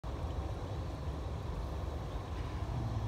ラビスタ霧島ヒルズのガーデンプール（2020年12月）
ラビスタ霧島ヒルズ宿の広大な敷地に大きなガーデンプールやドッグランなどあります。